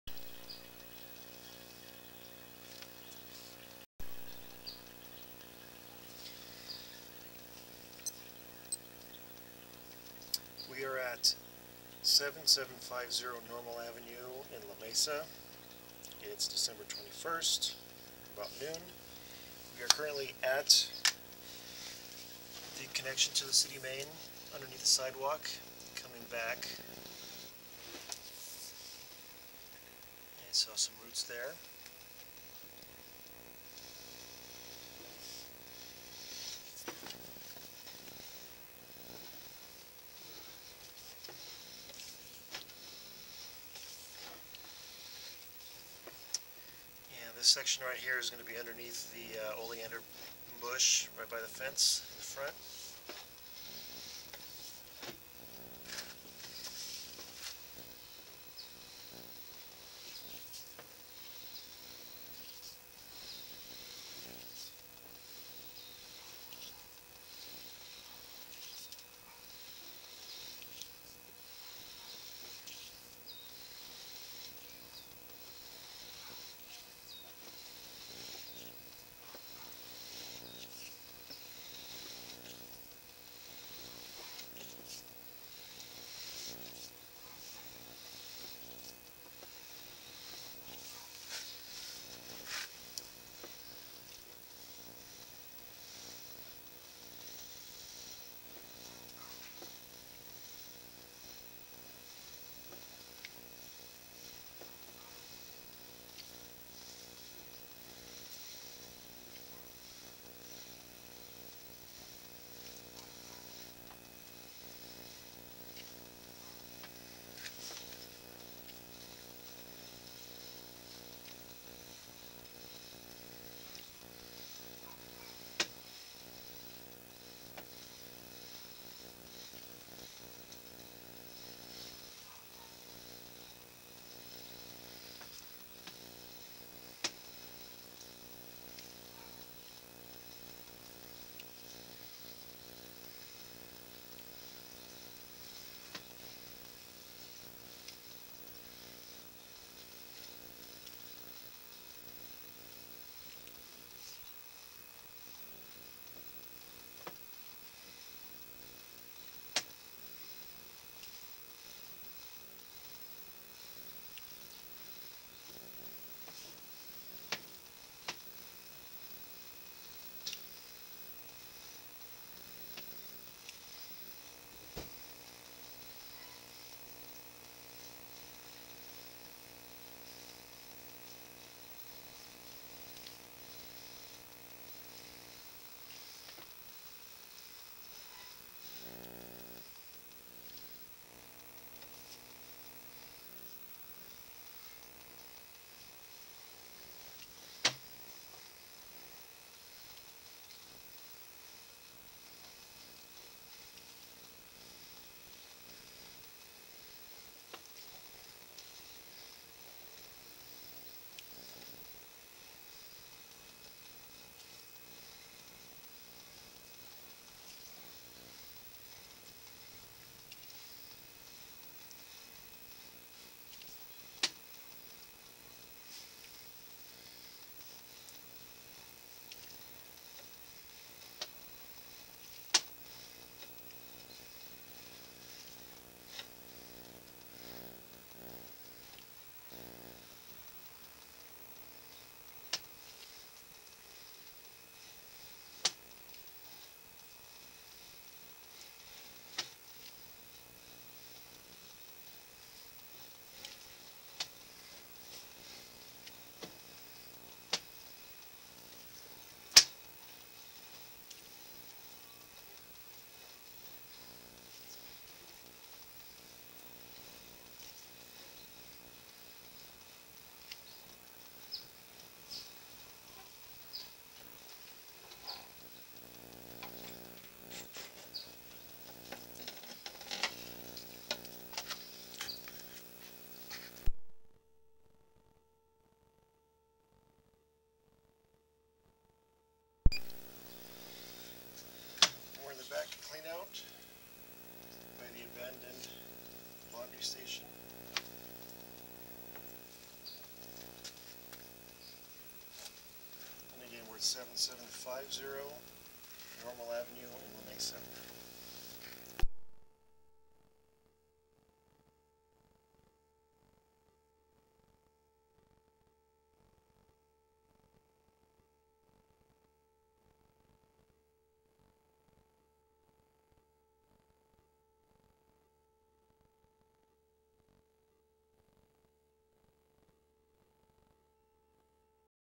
San Diego Property Drain Scope
Recently had a client of mine get a drain scoping.. I usually advise my buyers to get a drain scope if there may be a drain issue, especially when there are multiple units. Please let me if you could use my help in buying or selling a property.